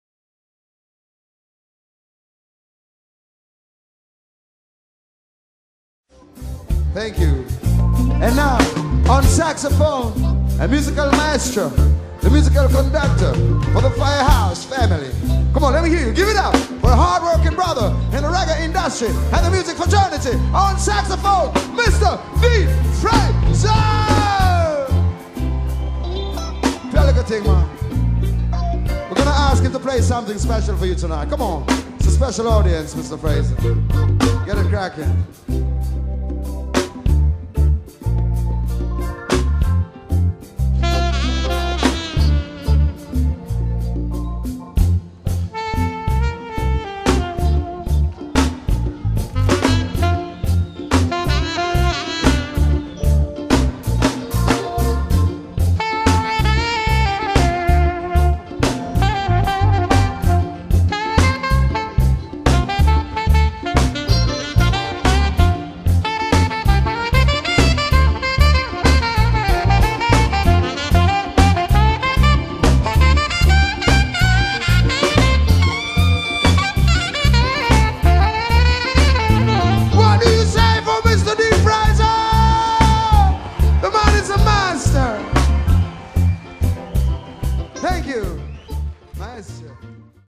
[Dean Frazer] saxo performance live in San Francisco (part2)
Dean Frazer, demonstration de son big big talent dans une petite impro ! Respect !!